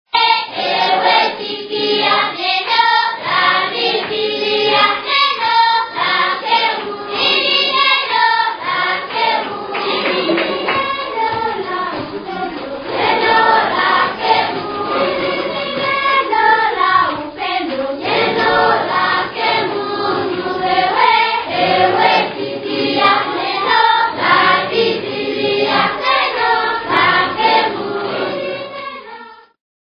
Kenyan students sing (November 2011)
In November 2011, Caritas Communications Officer Laura Sheahen visited Kenya to see the community response in action.